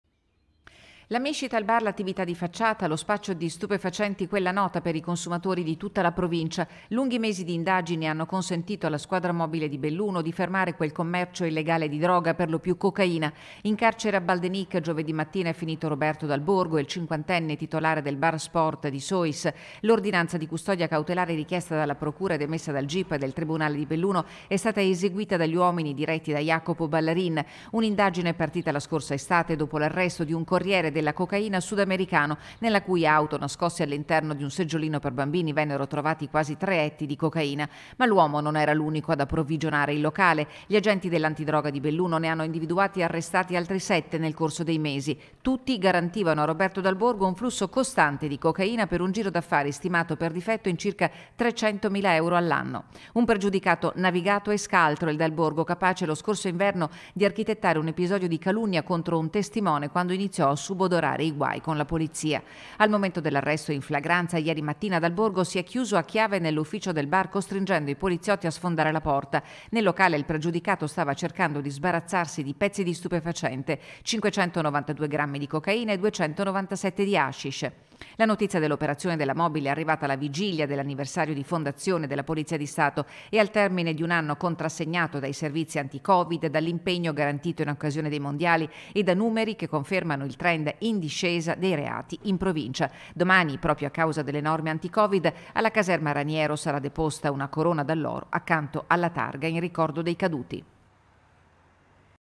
SPACCIAVA COCAINA AL BAR, ARRESTATO IL GESTORE | 09/04/2021
A3 NEWS Treviso 09/04/2021 - BELLUNO - Spaccio di cocaina, violazione di domicilio, lesioni personali aggravate e calunnie. E' finito di nuovo in carcere Roberto Dal Borgo, titolare del Bar Sois || La mescita al bar l'attività di facciata, lo spaccio di stupefacenti quella nota per i consumatori di tutta la provincia. Lunghi mesi di indagini hanno consentito alla Squadra Mobile di Belluno di fermare quel commercio illegale di droga, per lo più cocaina. In carcere a Baldenich giovedì mattina è finito Roberto Dal Borgo, il cinquantenne titolare del Bar Sport di Sois: l'ordinanza di custodia cautelare richiesta dalla Procura ed emessa dal Gip del Tribunale di Belluno è stata eseguita dagli uomini diretti da Jacopo Ballarin. Un'indagine partita la scorsa estate dopo l'arresto di un corriere della cocaina sudamericano nella cui auto, nascosti all'interno di un seggiolino per bambini, vennero trovati quasi 3 etti di cocaina. Ma l'uomo non era l'unico ad approvvigionare il locale: gli agenti dell'antidroga di Belluno ne hanno individuati e arrestati altri 7 nel corso dei mesi; tutti garantivano a Roberto Dal Borgo un flusso costante di cocaina per un giro d'affari stimato per difetto in circa 300 mila euro all'anno. Un pregiudicato navigato e scaltro il Dal Borgo capace lo scorso inverno di architettare un episodio di calunnia contro un testimone quando iniziò a subodorare i guai con la Poliza. Al momento dell'arresto in flagranza ieri mattina Dal Borgo si è chiuso a chiave nell'ufficio del bar costringendo i poliziotti a sfondare la porta: nel locale il pregiudicato stava cercando di sbarazzarsi di pezzi di stupefacente: 592 grammi di cocaina e 297 di hashish. La notizia dell'operazione della Mobile è arrivata alla vigilia dell'anniversario di fondazione della Polizia di Stato e al termine di un anno contrassegnato dai servizi anti Covid, dall'impegno garantito in occasione dei Mondiali e da numeri che confermano il trend in discesa dei reati in Provincia. Domani, proprio a causa delle norme anti Covid, alla caserma Raniero sarà deposta una corona d'alloro accanto alla targa in ricordo dei caduti. (Servizio di Tiziana Bolognani) - Segui Antenna Tre anche sul digitale terrestre! Visita il sito